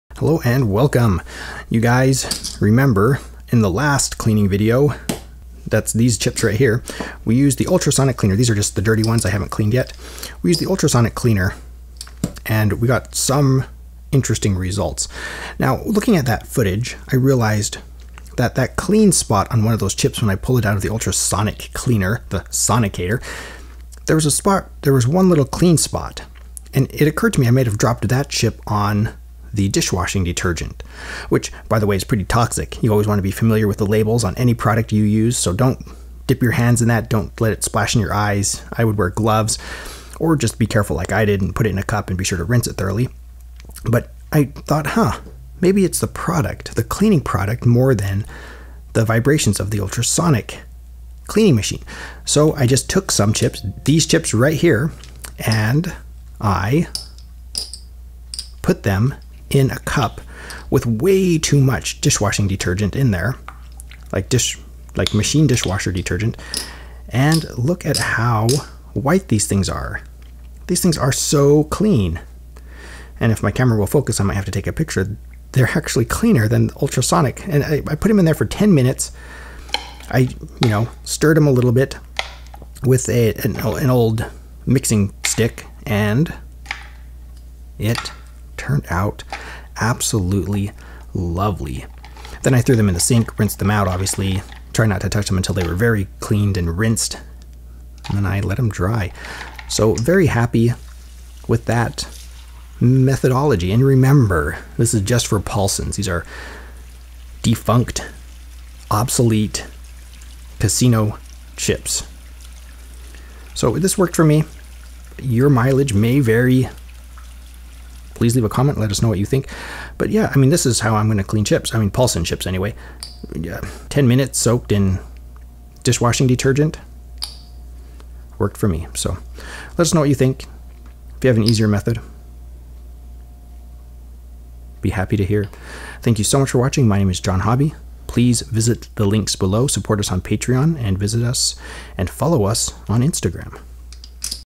Not A Video - Just A Cleaning Part 2
**NOTE** please be careful with the amount of product you use so you don't bleach or destroy your chips. Also the time in solution should also be watched carefully.

**Important** I would not try soaking any non-Paulson chip in a cleaning solution unless you are CERTAIN the label won't come out.

This is not a how-to video, just a documentary.

More cleaning trials.